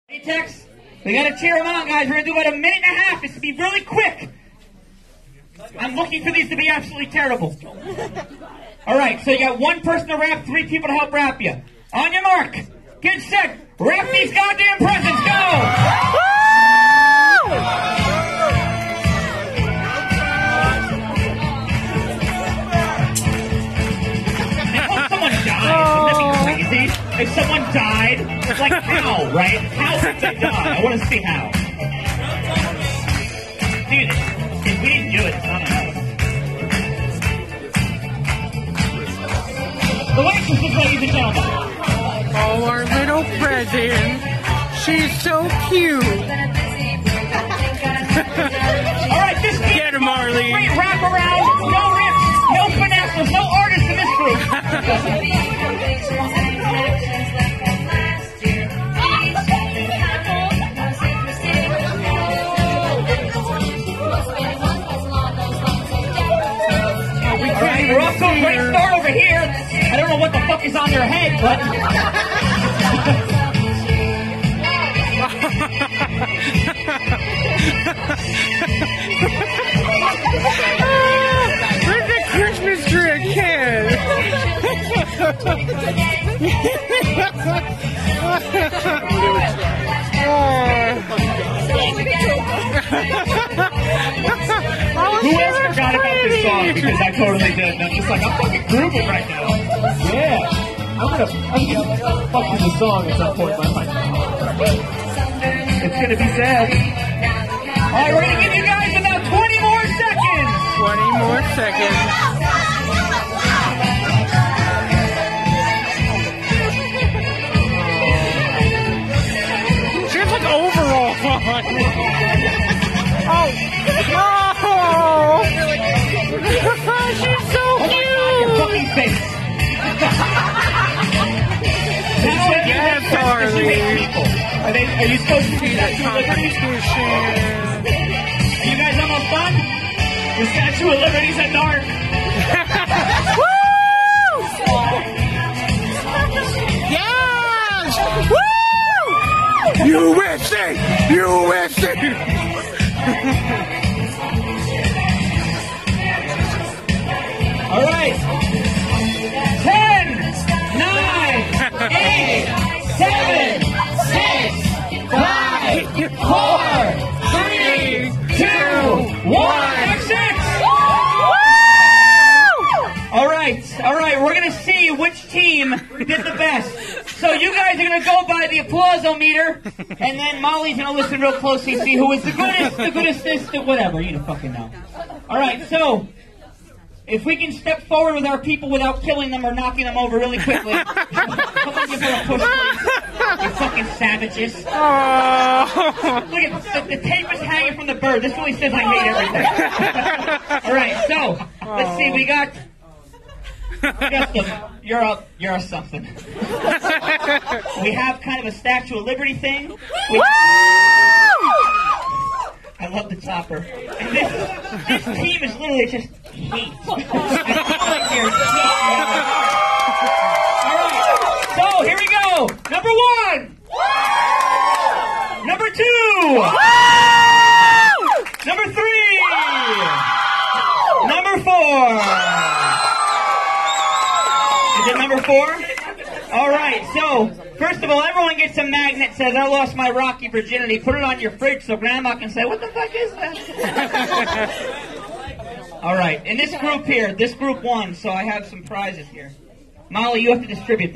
Arly's Virgin Ritual Part 2
On Friday Nite Specials in Aberdeen, NJ's Rocky Horror Picture Show Holiday Extravaganza!